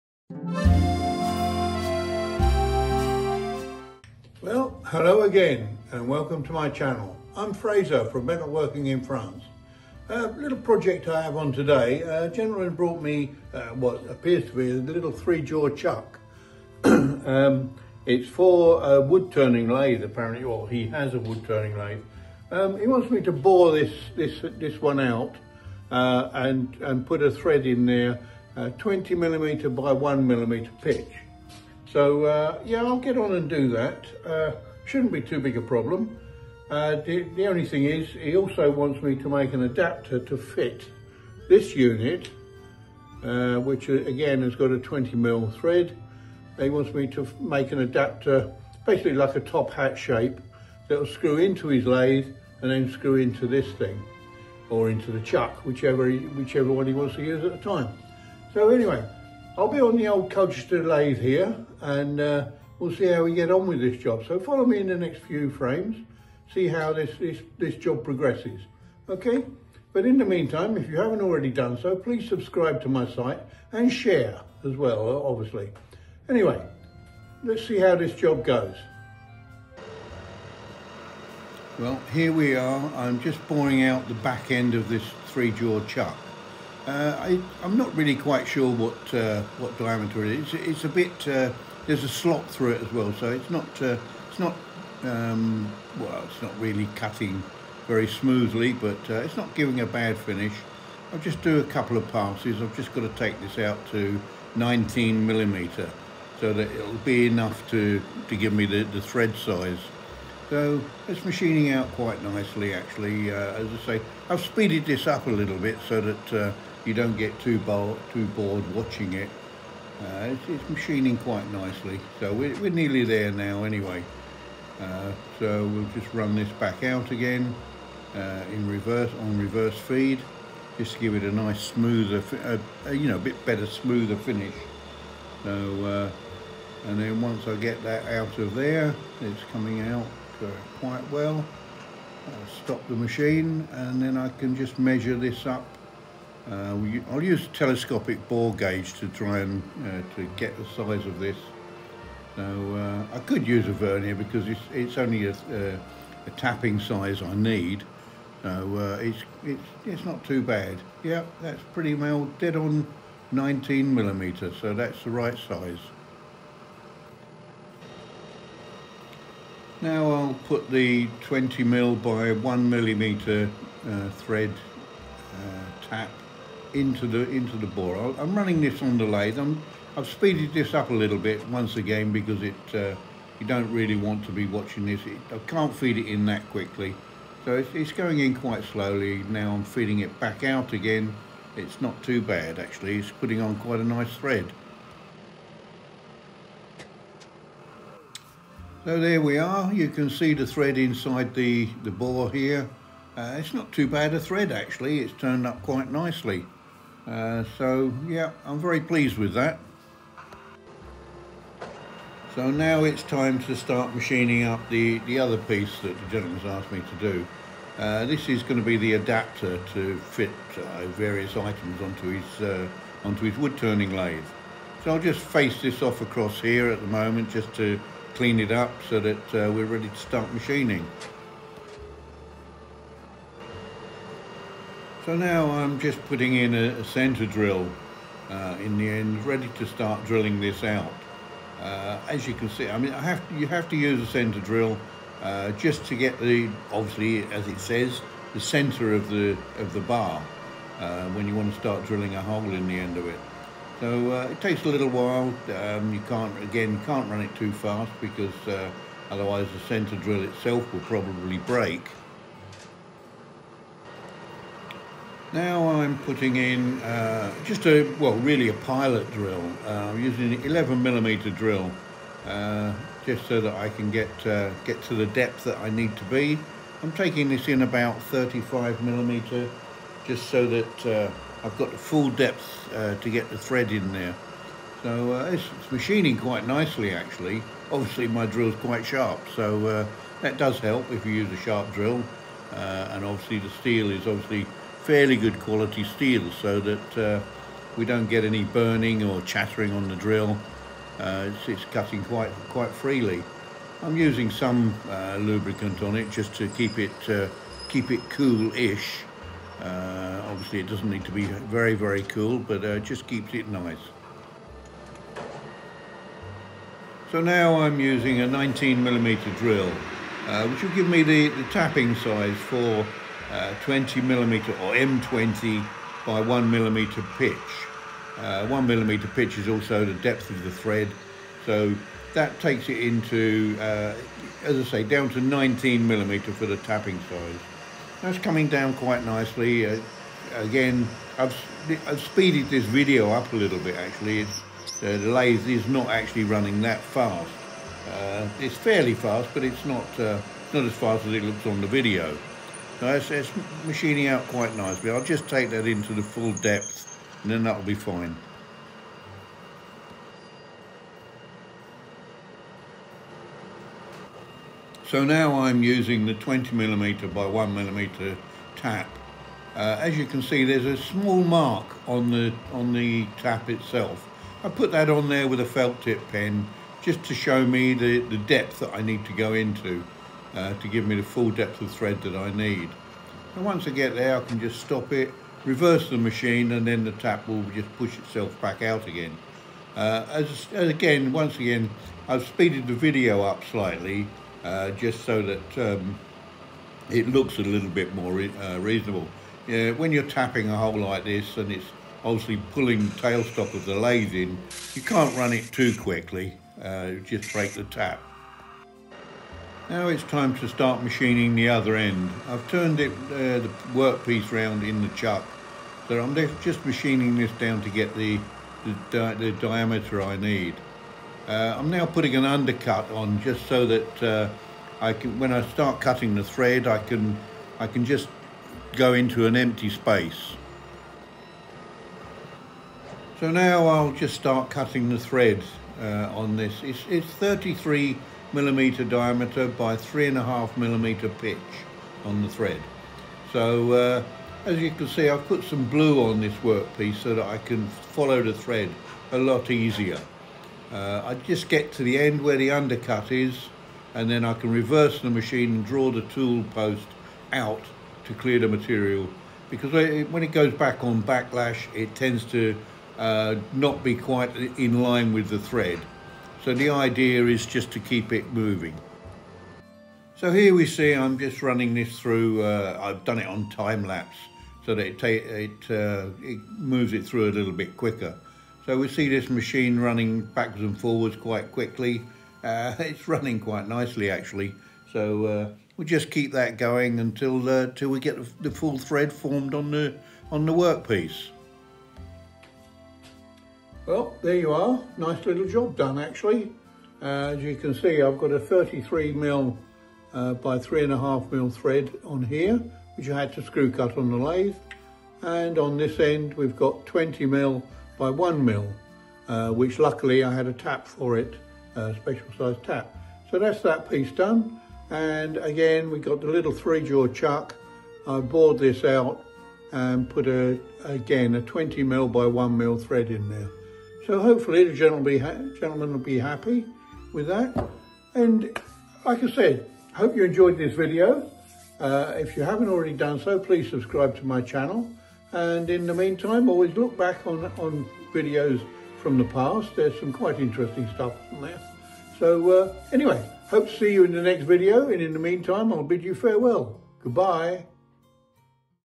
Modifying a 3 jaw chuck and making an adaptor
This video was made with Clipchamp, Machining a 3 jaw chuck & making an adaptor for a wood turning lathe